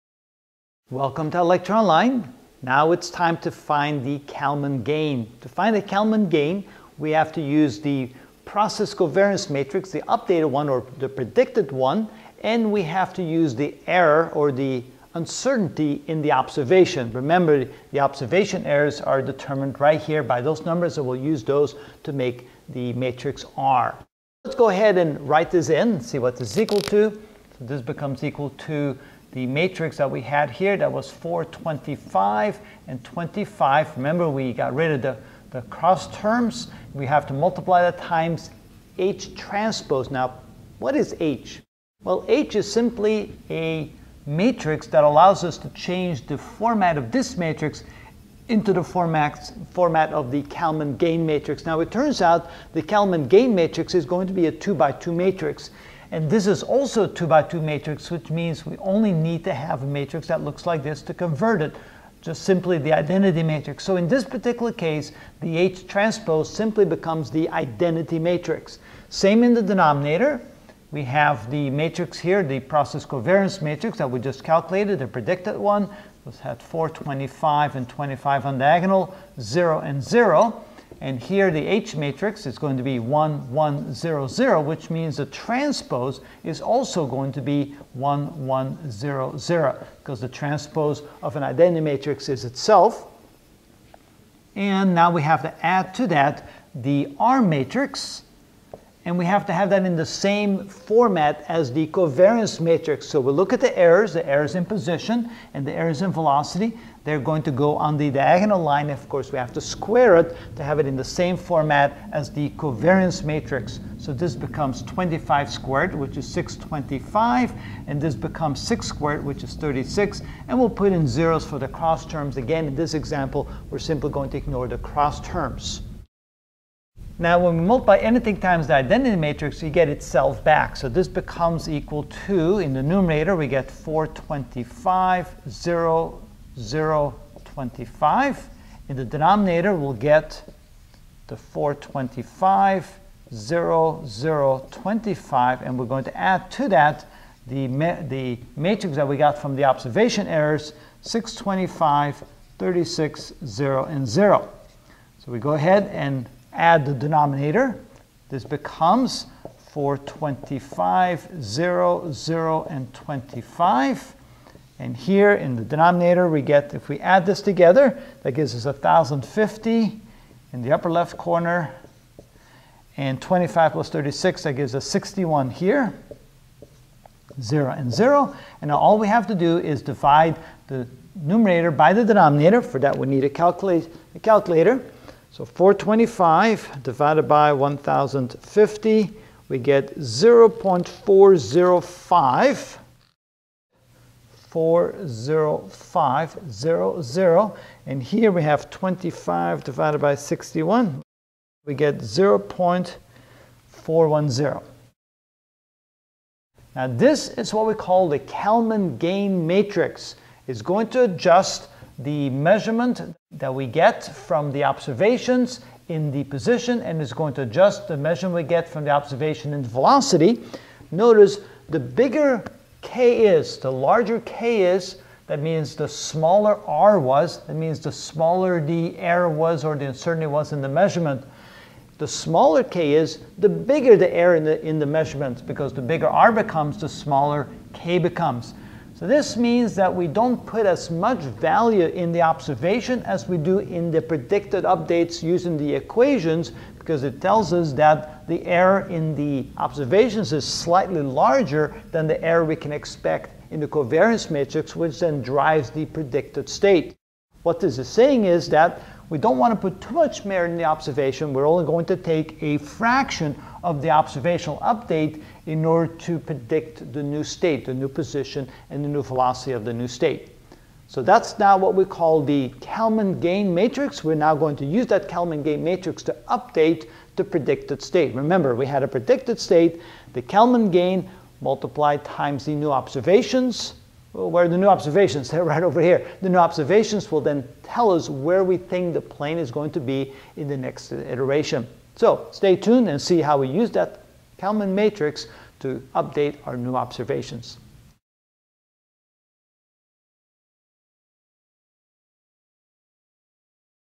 Special Topics - The Kalman Filter (30 of 55) 4. Calculate the Kalman Gain - Tracking Airplane
In this video I will calculate the Kalman Gain matrix of the Kalman Filter of tracking an airplane.